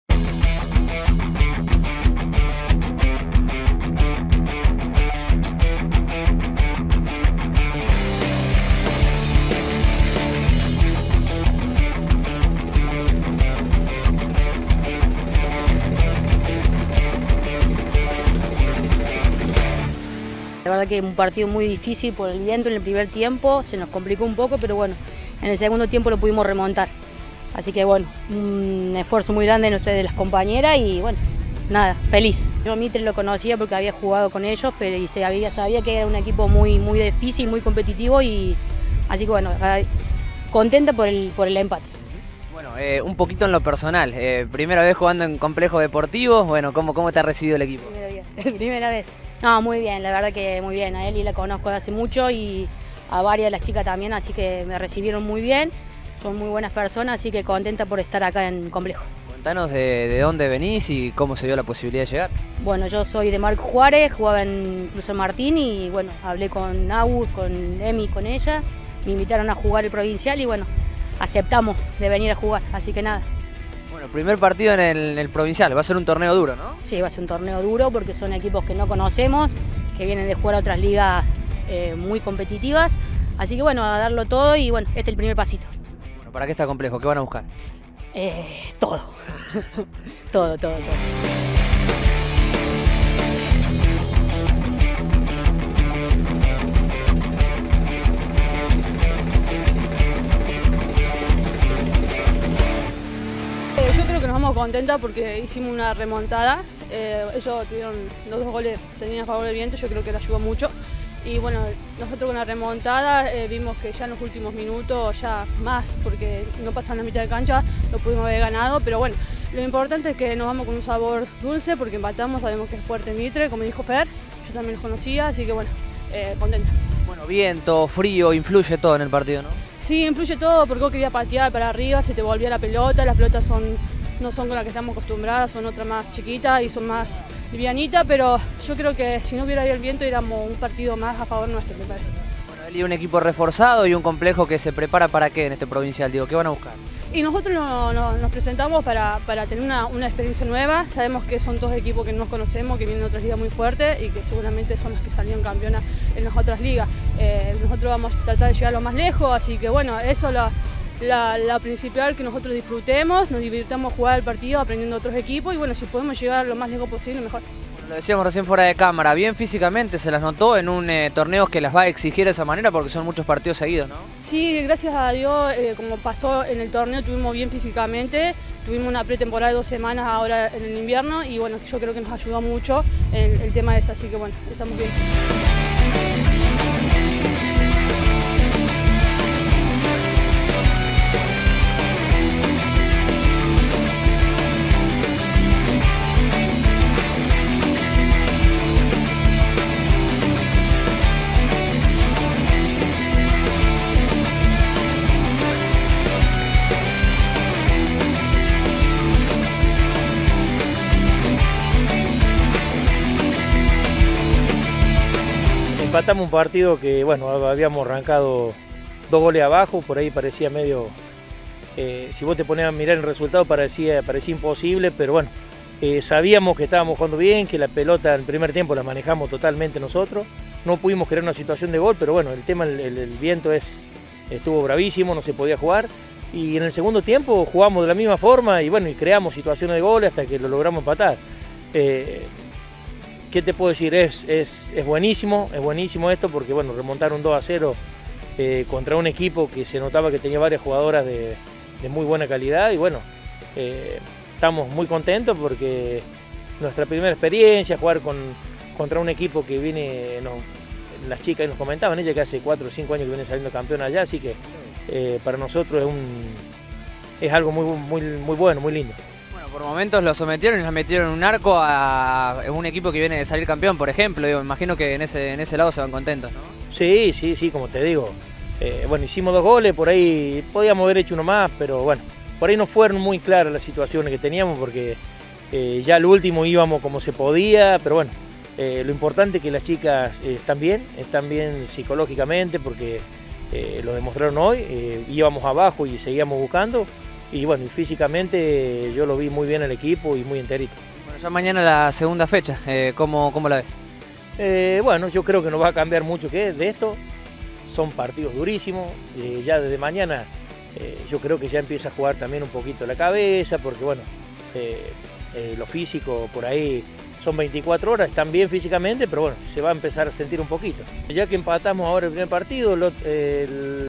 Torneo Provincial de Fútbol Femenino
Comenzó ayer en Bell Ville el primer torneo provincial de fútbol femenino de la historia. Justiniano Posse estará representado en Complejo Deportivo quien en el debut remontó un 0 - 2 para empatarlo y llevarse un punto de su primer presentación. Allí estuvimos y hoy presentamos el informe con imágenes y testimonios. Esta tarde, desde las 16:30, será el segundo partido de la C que enfrentará a Universitario de Villa María.